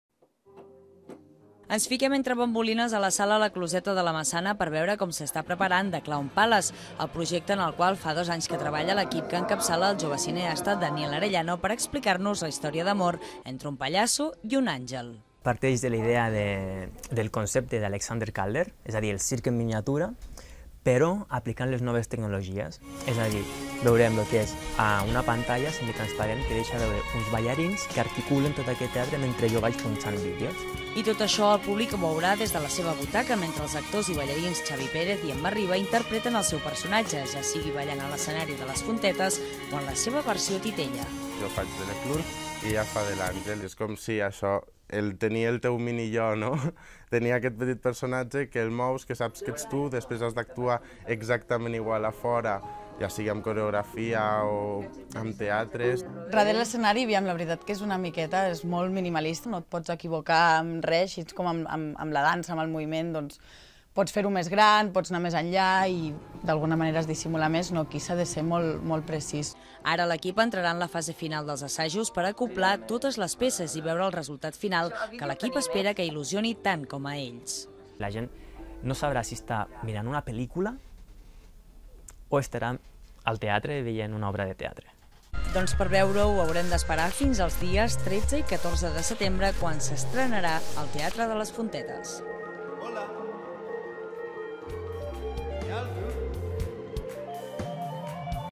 El jove cineasta Daniel Arellano recupera el circ en miniatura per a l'espectacle "Clown Palace"
Alexander Calder va idear el circ en miniatura, un projecte que ha recuperat el jove cineasta Daniel Arellano per crear un espectacle en el qual fa dos anys que treballa i que barreja diferents disciplines artístiques. Es titula 'Clown Palace' i ens explica la història d'un pallasso i un àngel que s'enamoren. S'estrenarà el 13 de setembre al Teatre de les Fontetes.

Ens fiquem entre bambolines a la sala La Closeta de la Massana per veure com s'està preparant 'Clown Palace', el projecte en el qual fa dos anys que treballa l'equip que encapçala el jove cineasta Daniel Arellano. Ens explica la història d'amor entre un pallasso i un àngel. "Parteix de la idea d'Alexander Calder, però aplicant les noves tecnologies amb una pantalla semitransparent que deixa veure uns ballarins que articulen el teatre mentre jo vaig projectant vídeos", manifesta Arellano.

I tot això el públic ho veurà des de la seva butaca, mentre els actors i ballarins Xavi Pérez i Emma Riba intepreten el seu personatge, ja sigui ballant a l'escenari de les Fontetes o en la seva versió titella. Pérez interpreta el pallasso i Riba l'àngel en una posada en escena que qualifiquen de molt precisa

En l'espectacle, que pretén trencar esquemes, hi han col·laborat uns músics del Canadà, que van conèixer fa uns anys al festival d'art de Reykjavík, i l'artista andorrana Eve Ariza, que ha creat els personatges de pasta, entre altres. Ara l'equip entrarà en la fase final dels assajos per acoblar totes les peces i veure el resultat final. Els artistes esperen que il·lusioni tant com a ells mateixos. Arellano assegura que "la gent no sabrà si està veient una pel·lícula o al teatre".

Per veure-ho, haurem d'esperar fins als dies 13 i 14 de setembre, en què l'obra s'estrenarà al Teatre de les Fontetes.